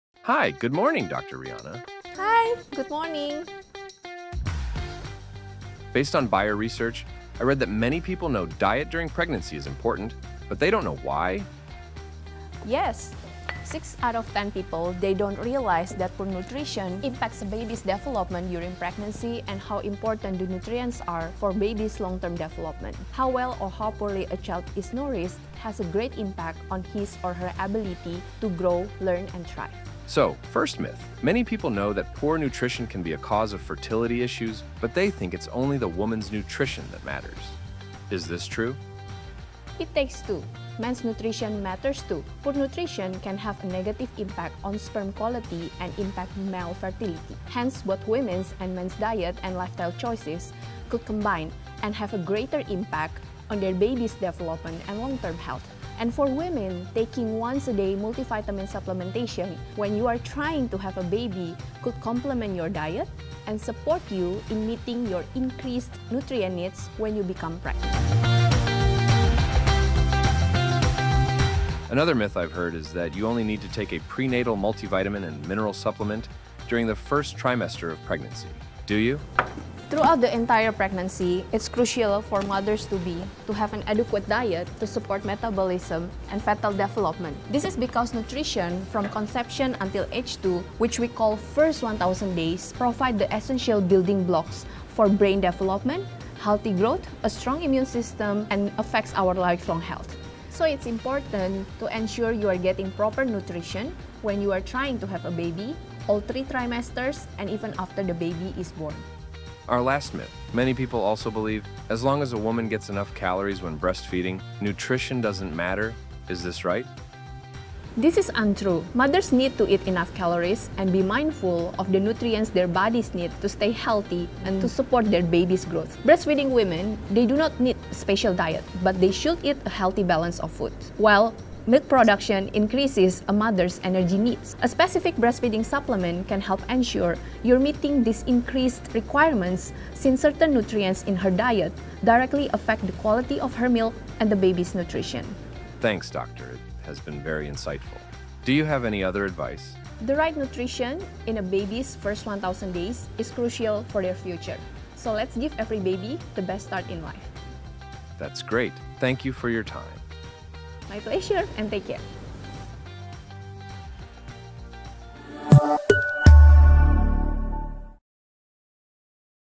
Prenatal Nutrition Myth Breaker
For 40 years, our brand Elevit, the world’s most clinically studied prenatal vitamin, has been dedicated to understanding the crucial role of nutrition from conception until age 2 for future health. We’re unveiling key findings from real-world data research that dispel common misconceptions about prenatal nutrition, providing valuable insights for both consumers and healthcare professionals. Together, we’ve supported millions of babies in having the best start in life – let’s continue this journey toward better health for all!